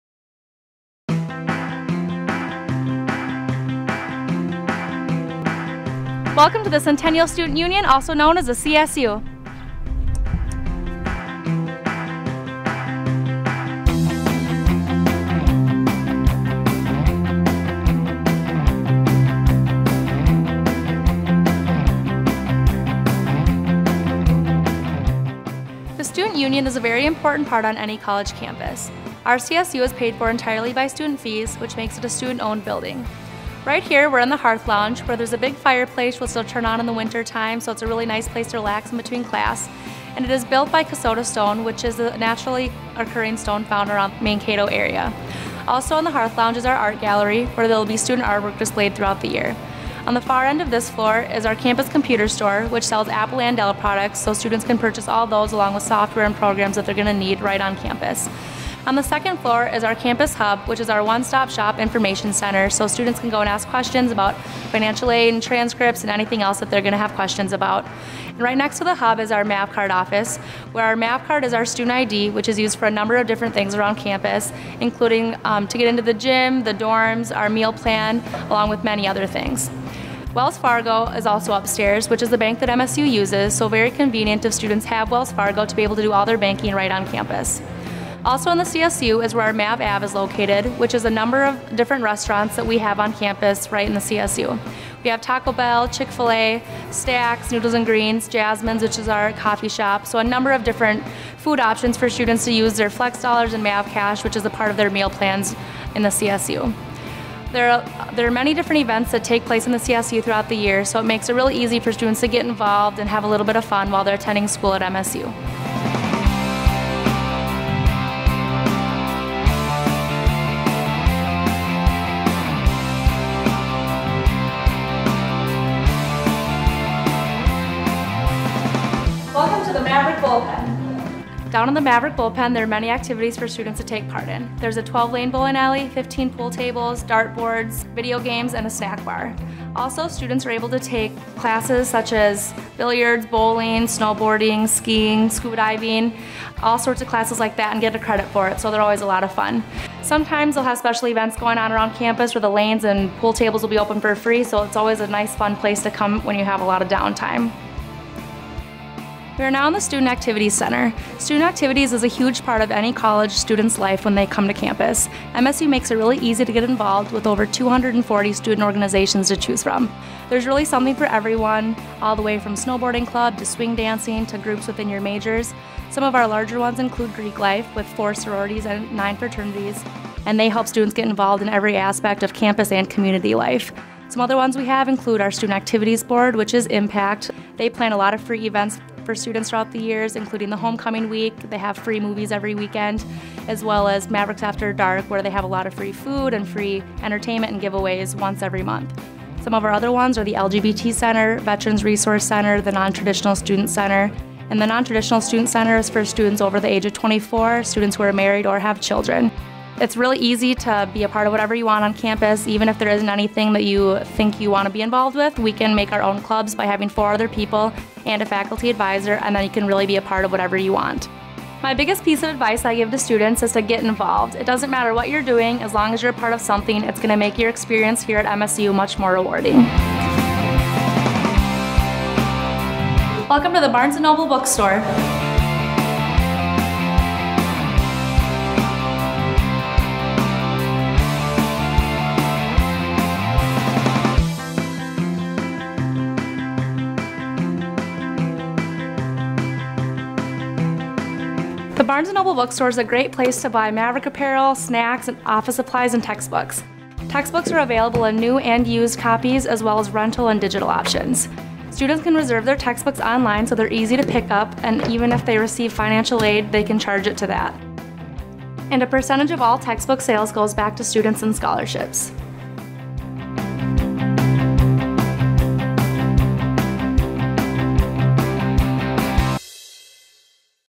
Campus Tour at Minnesota State, Mankato: Centennial Student Union
Situated in Southwestern Minnesota, the Minnesota State University, Mankato campus is known for it's vibrancy and beauty. See why so many people from all over the world choose to call Minnesota State Mankato home.